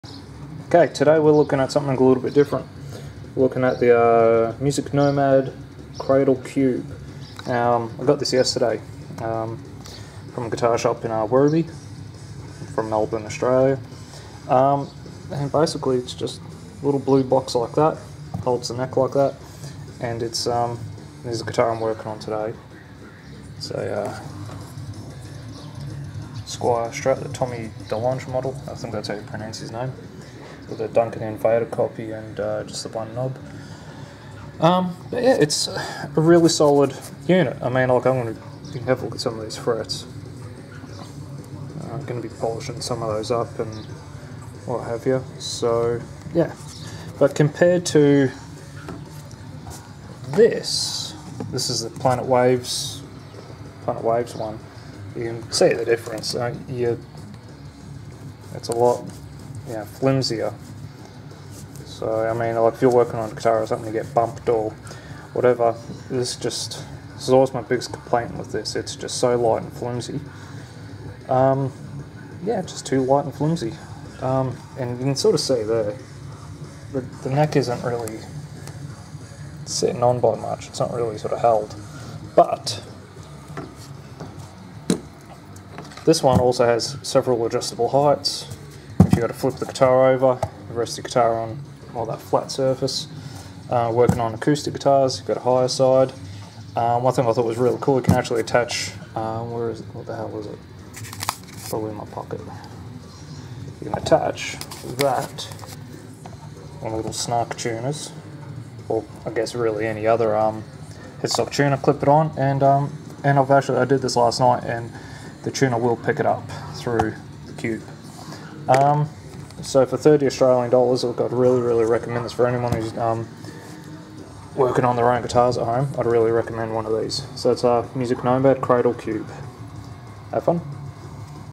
Music Nomad Cradle Cube
The Cradle Cube is one great way to support your neck on a variety of guitars, Id highly recommend if your doing work on any type of guitar! Sorry bout the shaky cam!